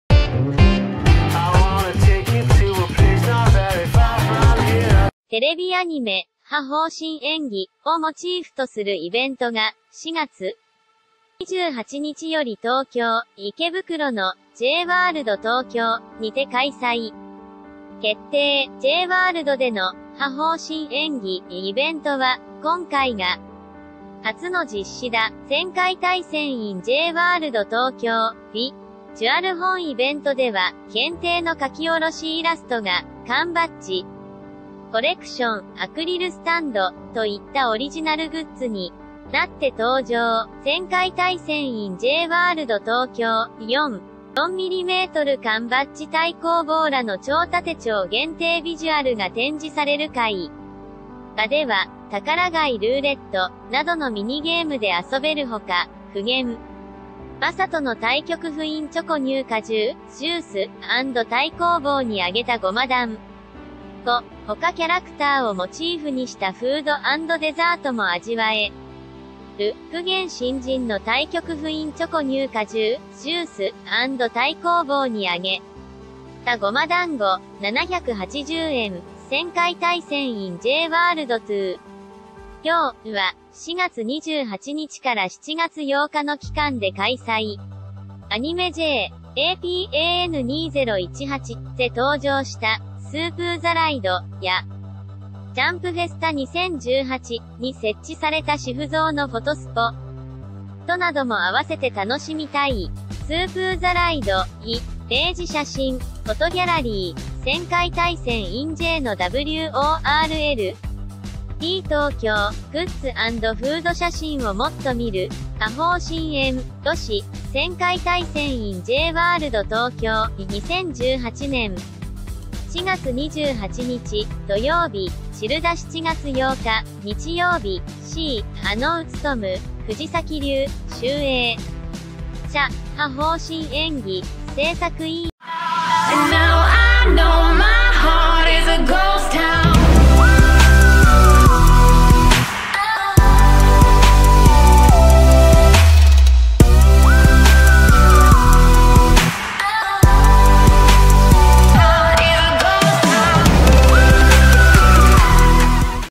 「覇穹 封神演義」J-WORLDで初イベント！ 描き下ろしグッズ、フード、"乗れる"スープーも！
TVアニメ『覇穹 封神演義』をモチーフとするイベントが、4月28日より東京・池袋の「J-WORLD TOKYO」にて開催決定。J-WORLDでの『覇穹 封神演義』イベントは、今回が初の実施だ。「仙界大戦 in J-WORLD TOKYO」ビジュアル本イベントでは、限定の描き下ろしイラストが「缶バッジコレクション」「アクリルスタンド」といったオリジナルグッズになって登場。「仙界大戦 in J-WOR...